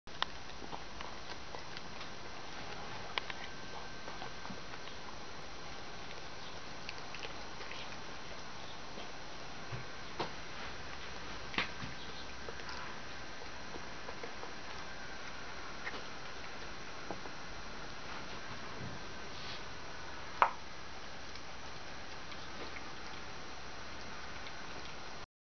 ガム、欲しいんだけど・・
自分は早々と食べ終わって、ほかの子のも欲しくてたまらない。
でも、じっと待っています。